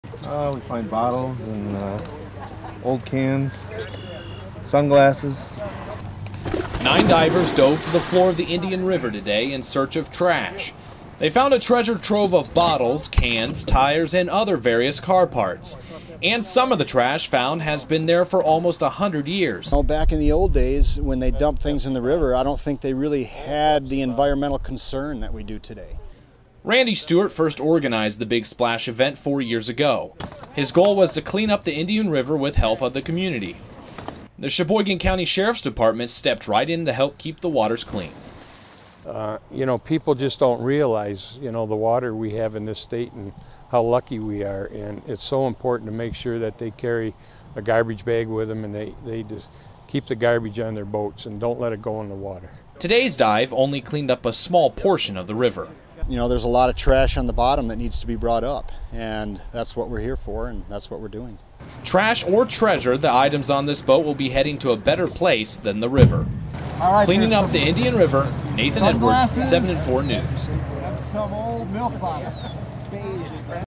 Treasure Trove of Trash cleaned up in the Indian River
Dive teams cleaned up the Indian River in Cheboygan County.